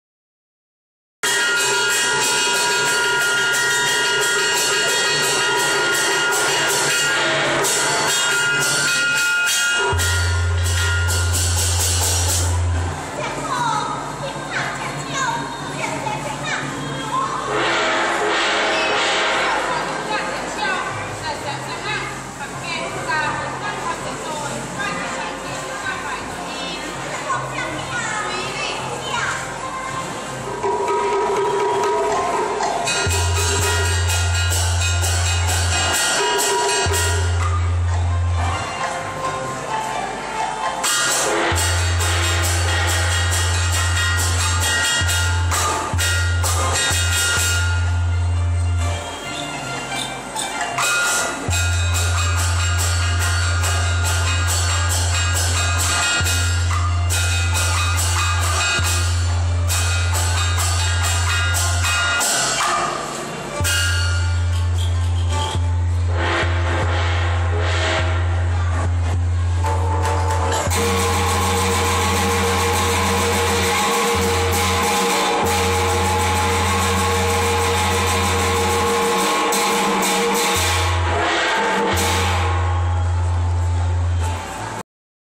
Chinese Opera in Maha Sarakham, Thailand (2011) 2/2
Chinese Opera Performance at a Chinese Temple during an annual Chinese festival in Maha Sarakham City, Northeast Thailand